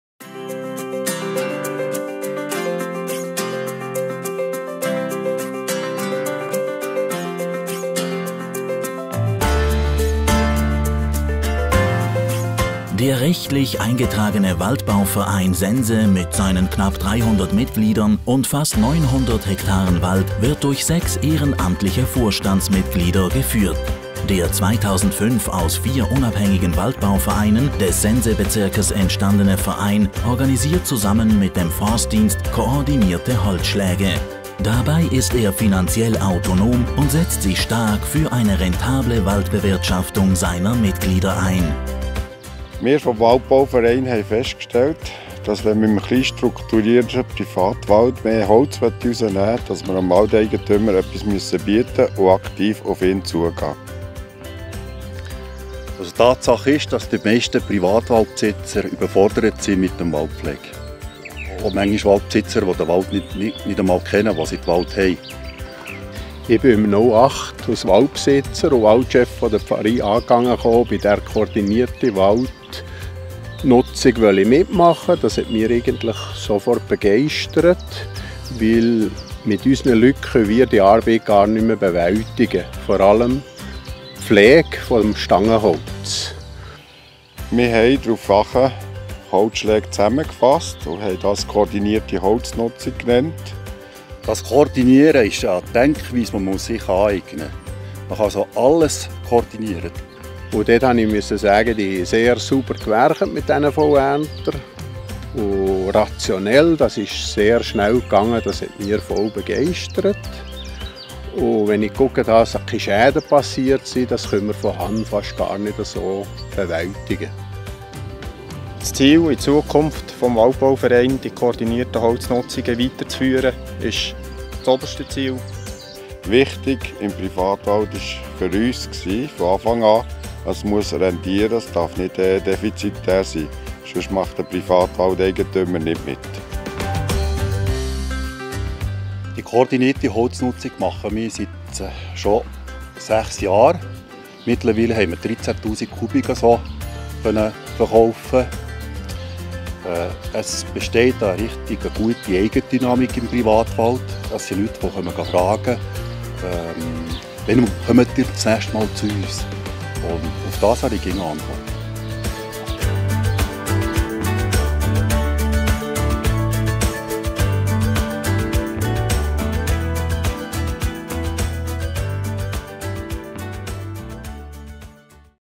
Kooperationen Waldbauverein Sense
Der Waldbauverein Sense ist ein Beispiel einer erfolgreichen forstlichen Zusammenarbeit. Die Beteiligten erzählen im Film von ihren Erfahrungen und erläutern die Zusammenarbeit. Ein Kurzfilm im Rahmen des Projekts von WaldSchweiz "Kooperationen im Wald - Zäme zum Wald luege".